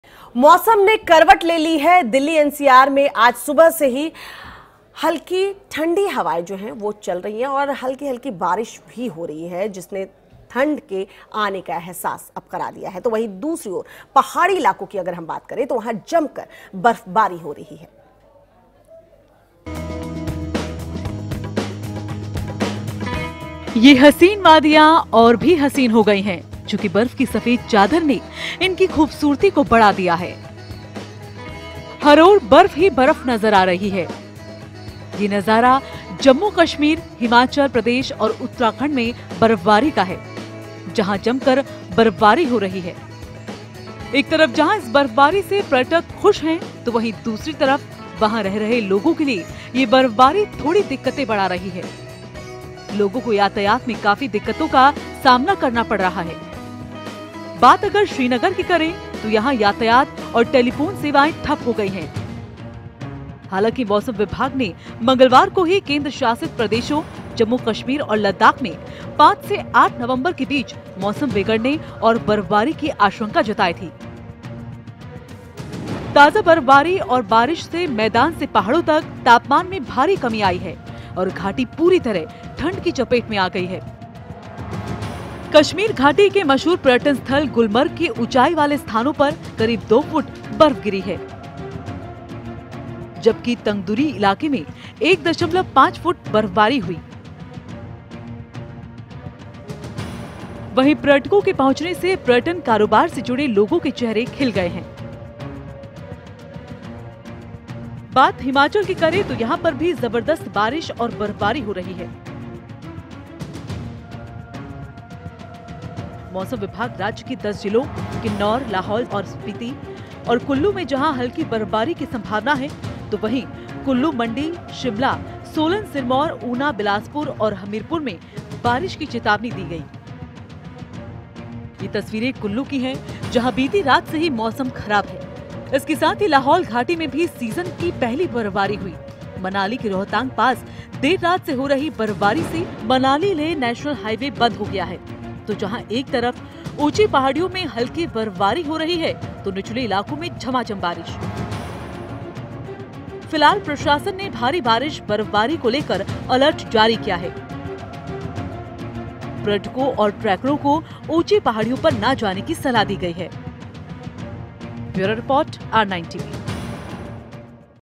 मौसम ने ली करवट...Delhi-NCR में चली ठंडी हवाएं
मौसम ने करवट ले ली है...दिल्ली -एनसीआर में आज सुबह से ही हल्की ठंडी हवाएं चल रही है..जिसने थंड के आने का अहसास दिला दिया है...तो वहीं दूसरी ओर पहाड़ी इलाकों में जमकर बर्फबारी हो रही है ..